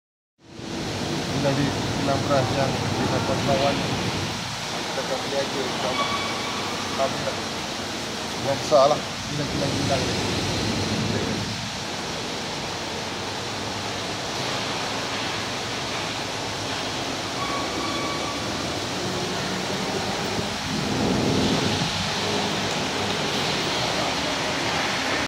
KILANG BERAS MILIK RAKAN MADU3 DI THAILAND
LAWATAN ke  KILANG BERAS  dan GULA MILIK RAKAN MADU3 BANGKOK  DI THAILAND

SESIAPA NAK BELI BERAS DAN GULA DARI THAILAND BOLEH LAH

HUBUNGI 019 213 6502 irsyad sidi kumpulan perniagaan MADU3